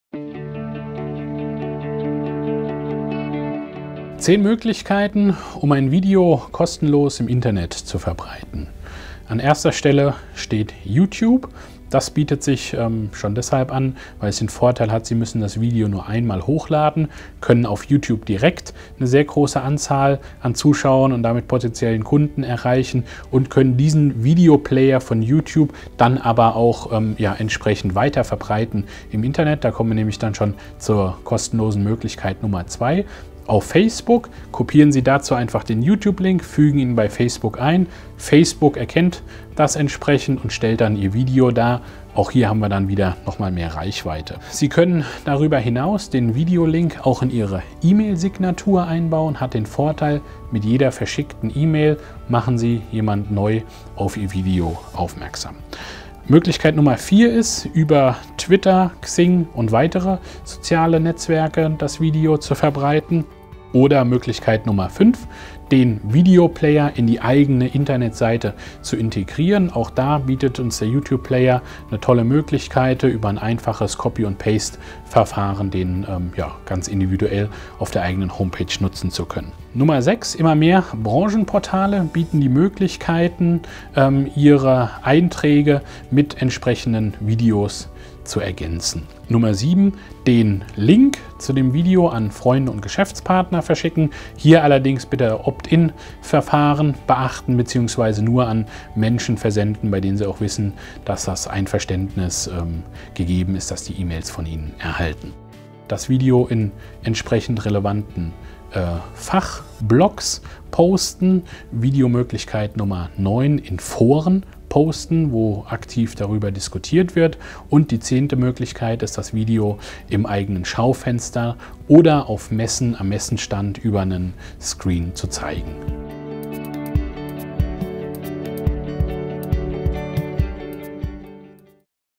10 Verbreitungstipps - Videomarketing - Videoproduktion-bundesweit.de
Videos lassen sich einfach und kostenlos verbreiten um damit Reichweite zu erzielen.